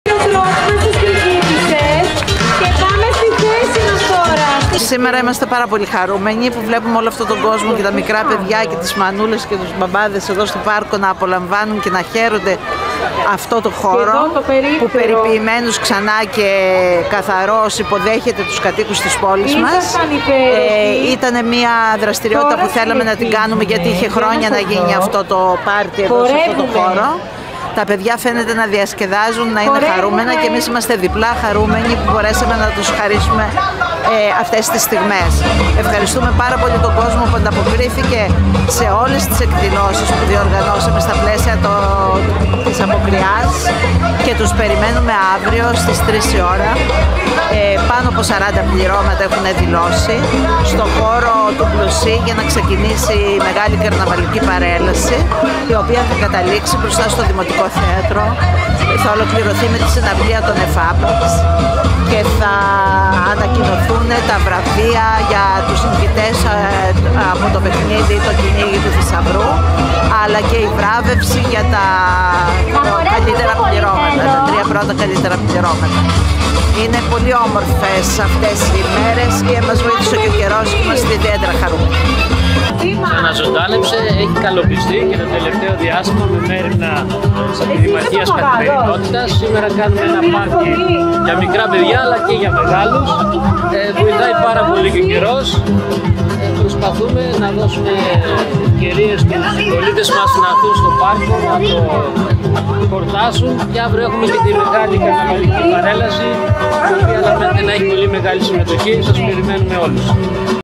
Το «Καρναβάλ’» Μυτιλήνης σε πλήρη εξέλιξη!
Με παιδικό αποκριάτικο πάρτι στο Πάρκο της Αγίας Ειρήνης ξεκίνησε το Καρναβαλικό διήμερο στη Μυτιλήνη.